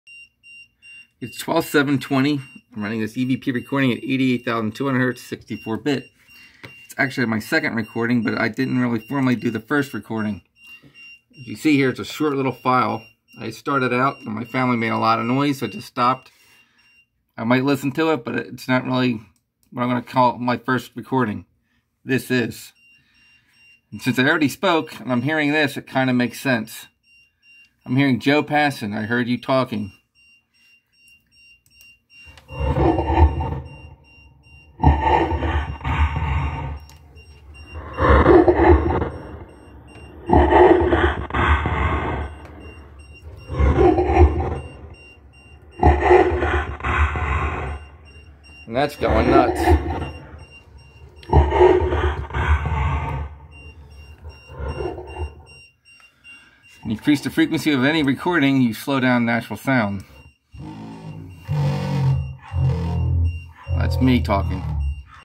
1. EVP – “Joe Passon I heard you talking” was spoken
My first recording tonight is actually my second after a brief start and stop because of noisy kids and a wife. I started over and counted this recording as my first. I show the save file in the list and since I already called out this EVP makes perfect sense to me.